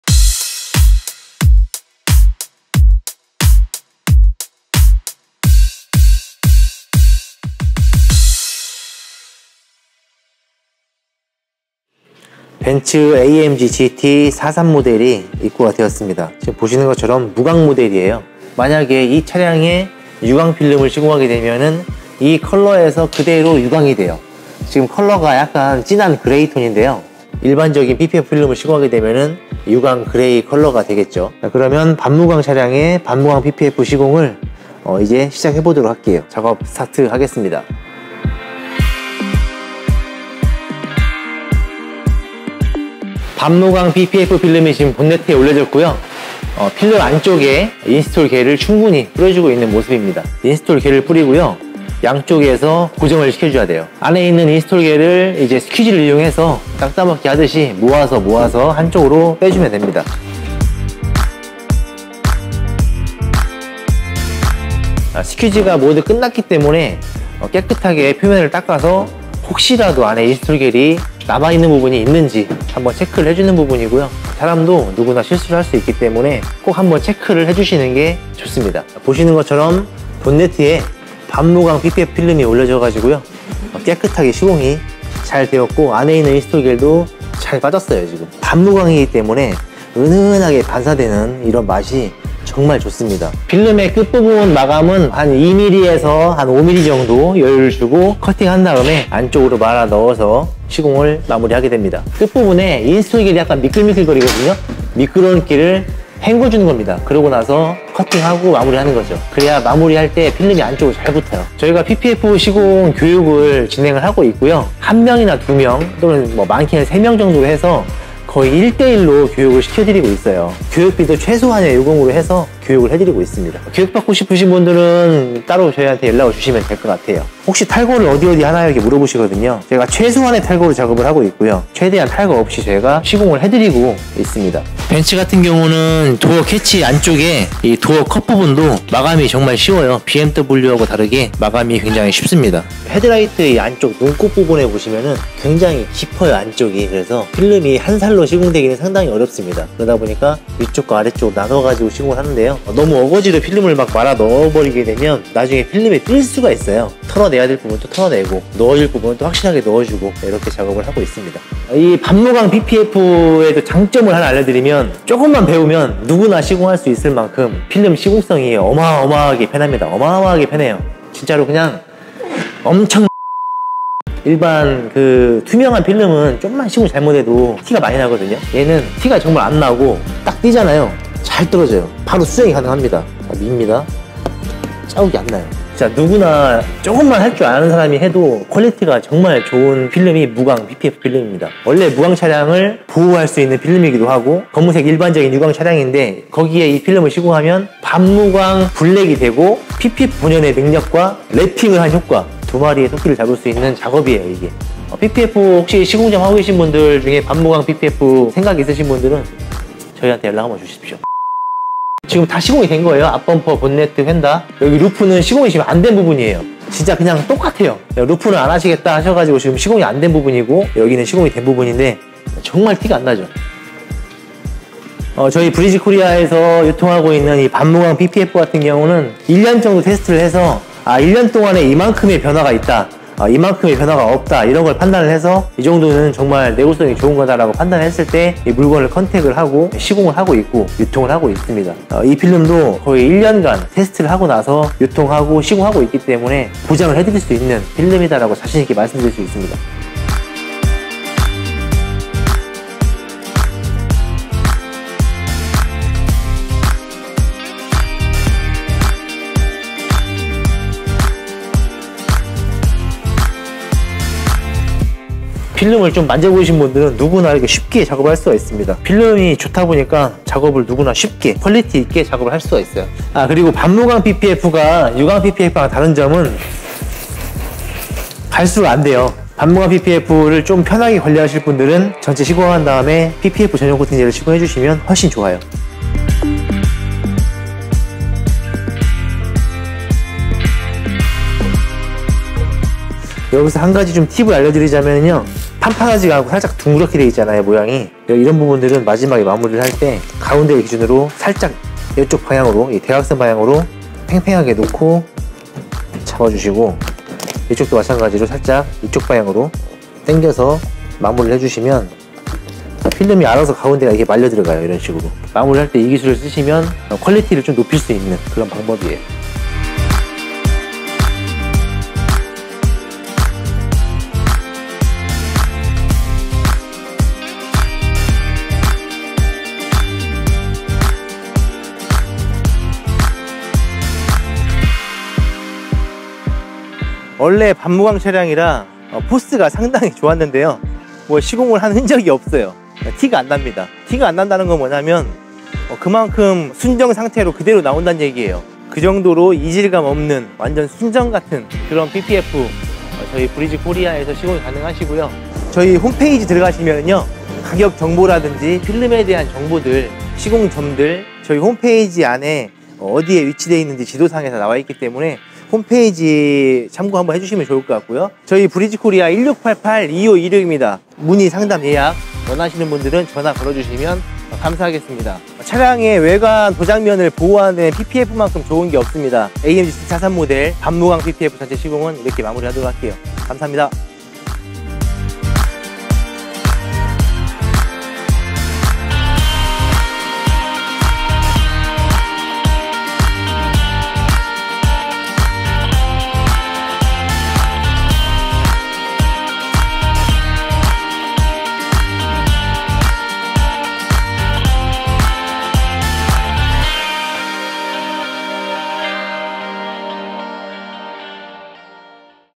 무광PPF 하나로 랩핑과 PPF 효과를 한 방에!!!│벤츠 AMG GT43
[ 벤츠 AMG GT43 반무광 전체PPF ]

안녕하세요. 고퀄리티 시공을 지향하는 브리즈코리아 입니다.

* Nano-Fusion PPF전문점 모집
* 세라믹 언더코팅 취급점/시공점 모집
* 가맹 및 창업문의
* 시공교육 문의

*시공문의/예약문의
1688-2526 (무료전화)